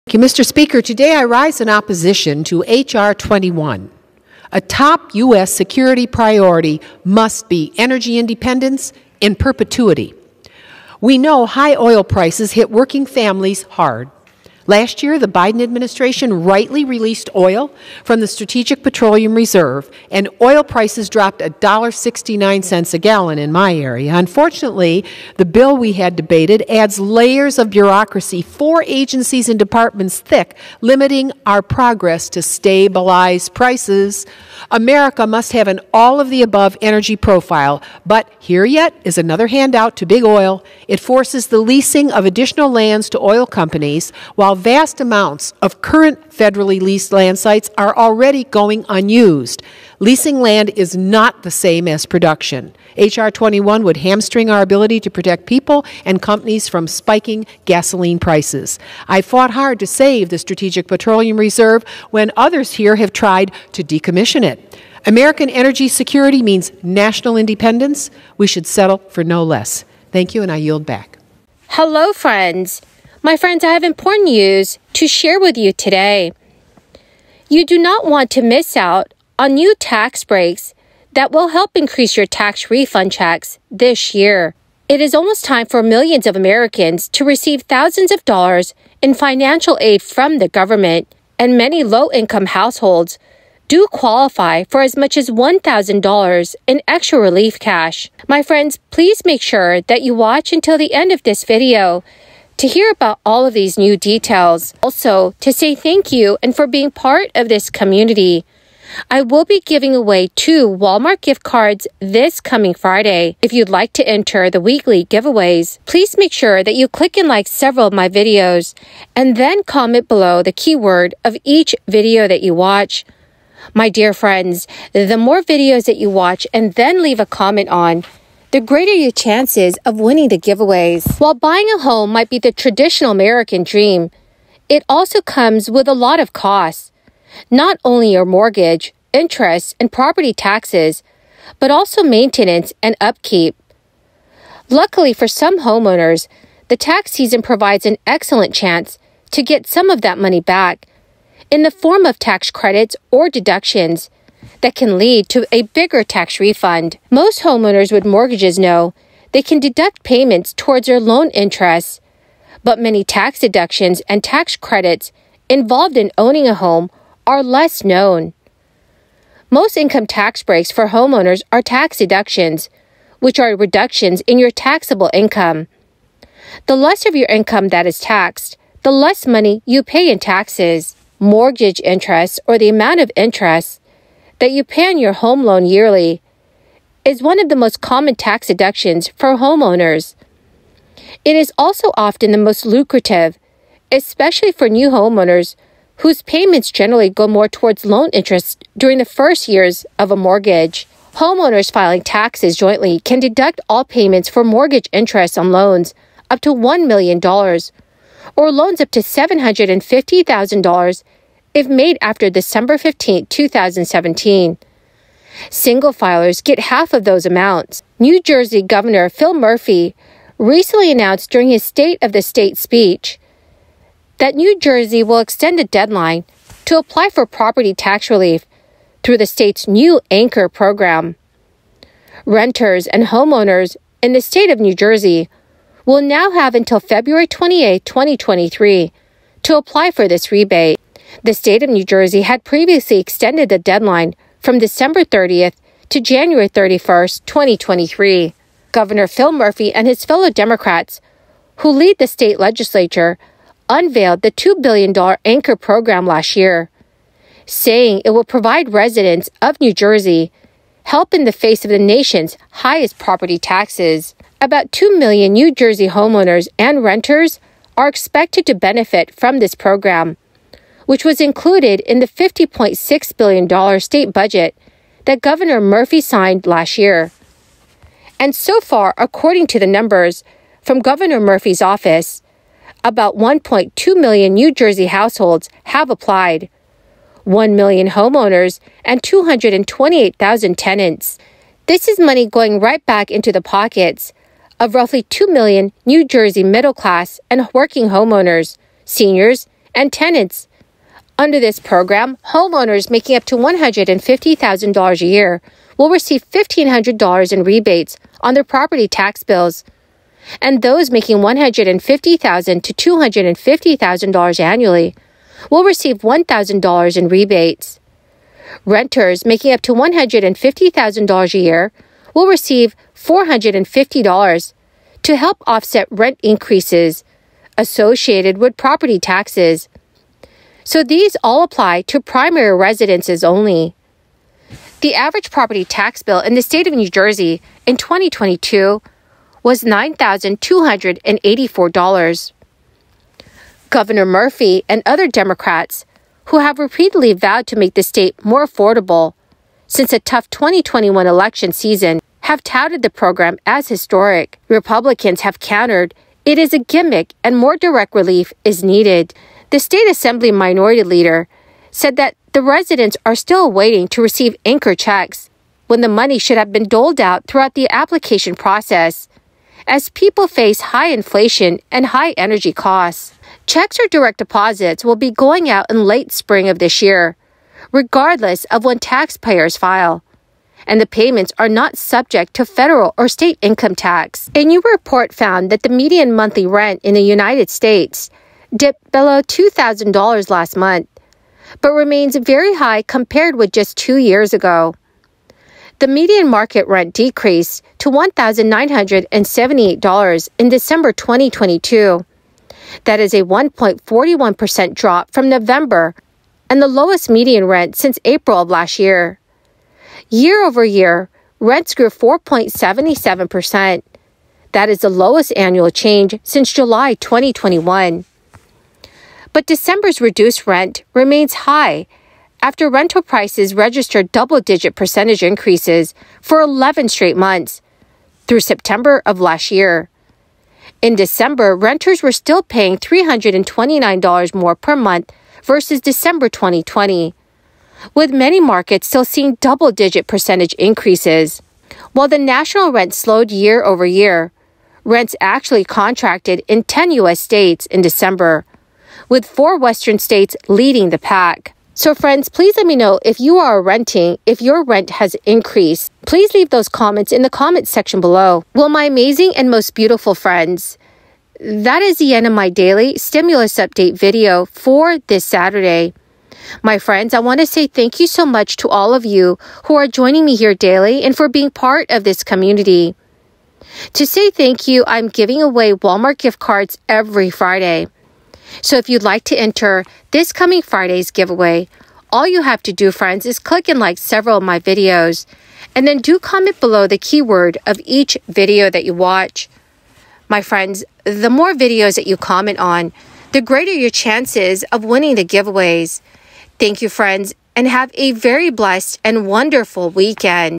CHECK YOUR BANK ACCOUNT & MAILBOX! DEMOCRATS SAID THIS CHECK WILL BE SENT BY IN FEBRUARY!
This is your fourth stimulus check update, fourth stimulus package update, and news report for January 29th! In this video, I am going to share the latest stimulus news with you all, as well as the status of the fourth stimulus check!

stimulusupdate​​​​​​​ stimuluspackage​​​​​​​ ​​stimuluscheckupdate​​​ Jesus loves you! God bless you!